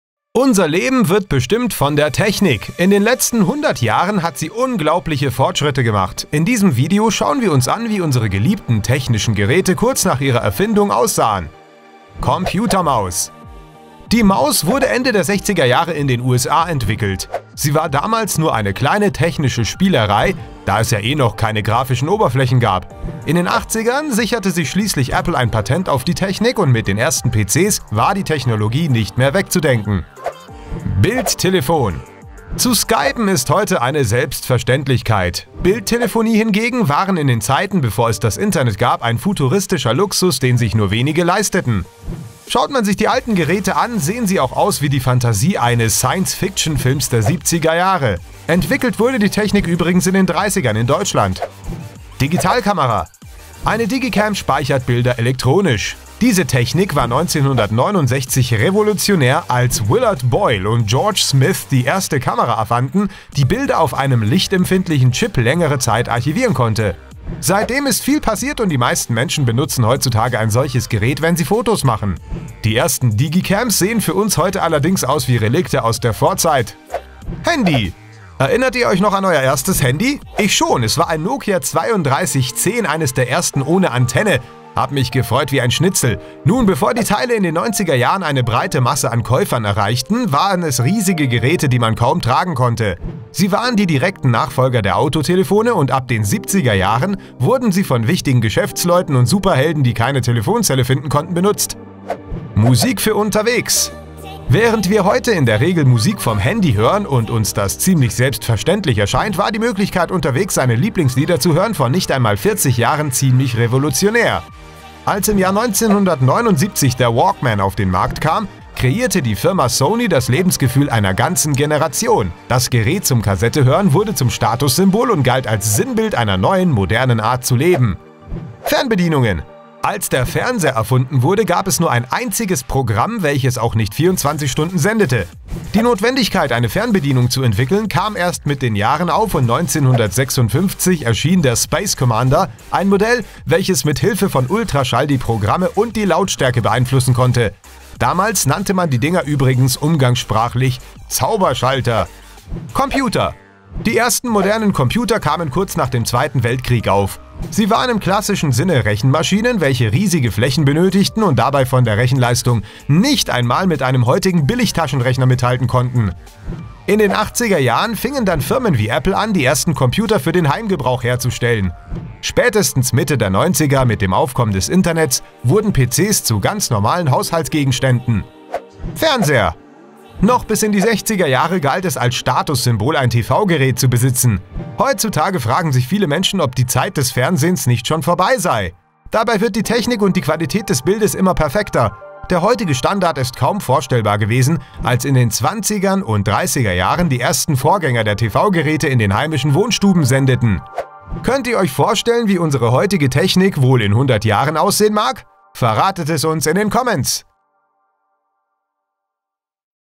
Technik Früher vs. Heute
Unser Leben wird bestimmt von der Technik. In den letzten 100 Jahren hat sie unglaubliche Fortschritte gemacht. In diesem Video schauen wir uns an, wie unsere geliebten technischen Geräte kurz nach ihrer Erfindung aussahen.

➡ Faktastisch "Tierwelt"-Buch auf Amazon
➡ Faktastisch "Liebe"-Buch auf Amazon

Schicke uns Verbesserungsvorschläge oder Ideen für den YouTube-Kanal an: kontakt(aet-Zeichen)faktastisch.net